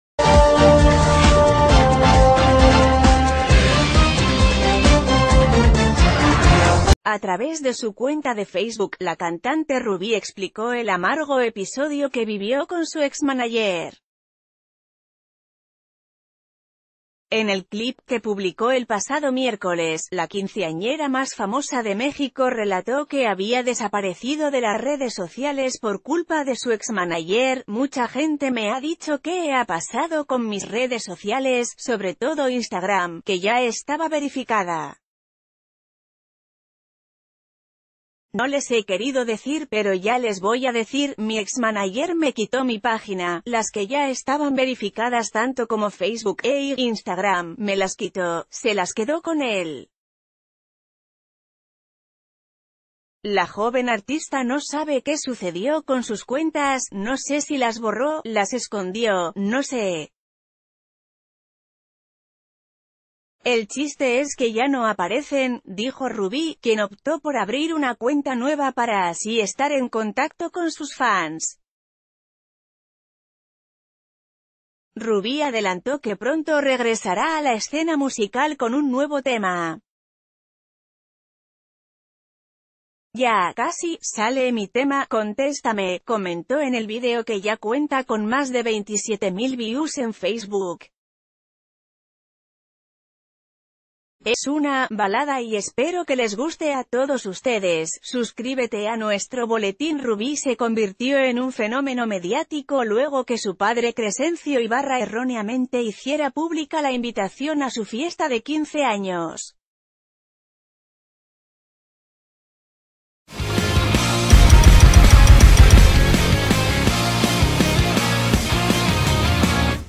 Rubí Ibarra reaparece en redes y revela amargo episodio que vivió con su exmanager
A través de su cuenta de Facebook, la cantante Rubí explicó el amargo episodio que vivió con su exmanager. En el clip que publicó el pasado miércoles, la quinceañera más famosa de México relató que había desaparecido de las redes sociales por culpa de su exmanager: “Mucha gente me ha dicho qué ha pa...